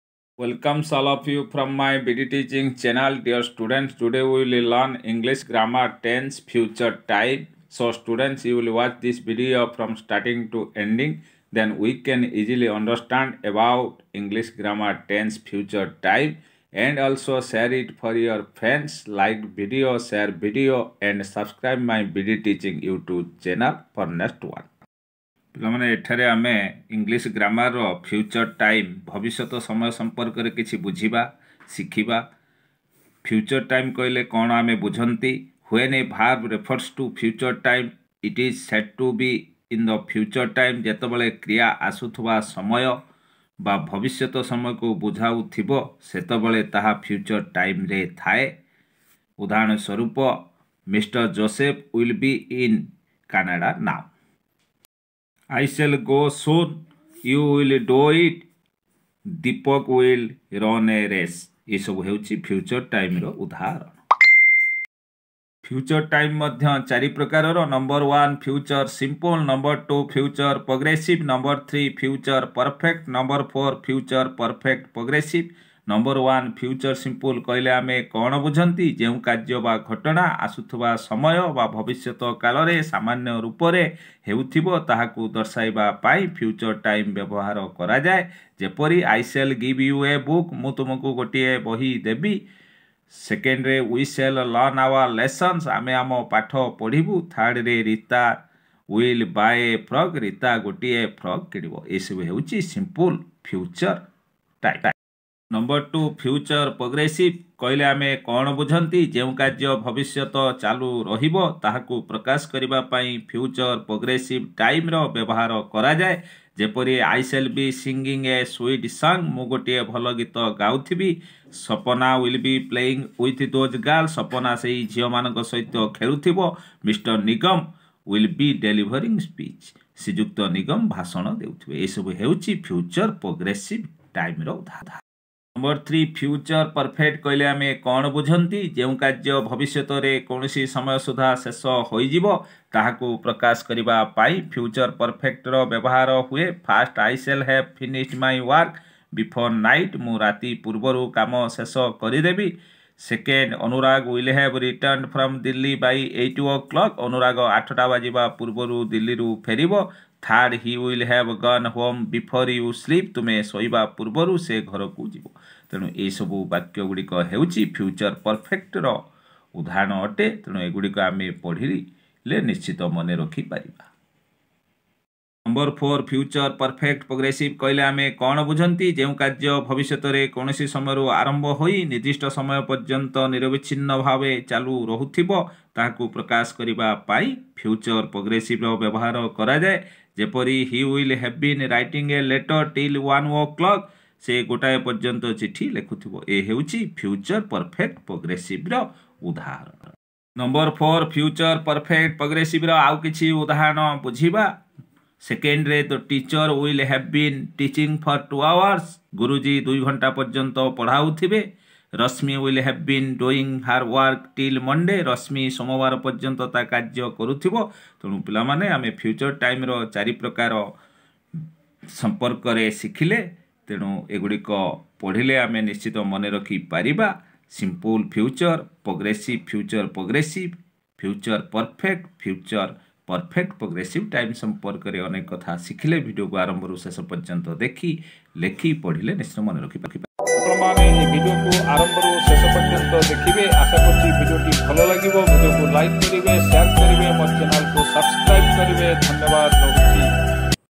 English Grammar -Tense Future Time and 4Types. Easy Process.
English Grammar -Tense
Future Time and  Its 4 Types.
Easy Process and discussion.